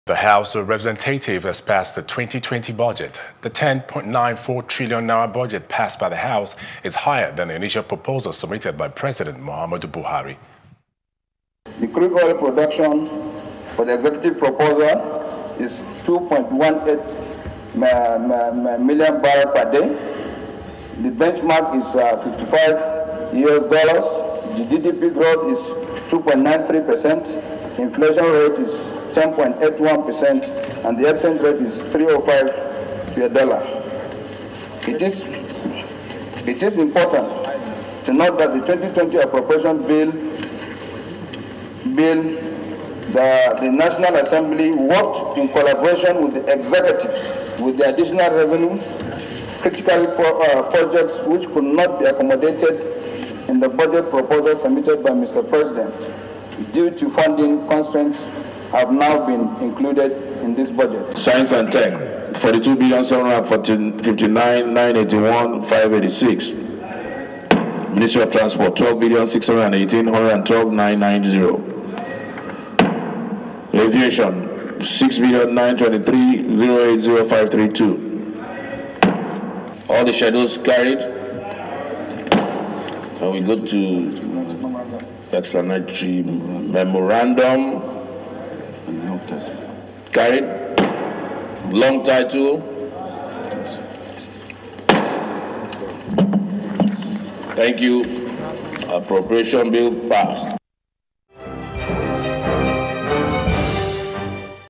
2020 Appropriation Bill: House Of Representatives Passes Budget
The House of Representatives has passed the twenty-twenty budget.

The ten point nine-four trillion-naira budget passed by the House is higher than the initial proposal submitted by President Muhammadu Buhari.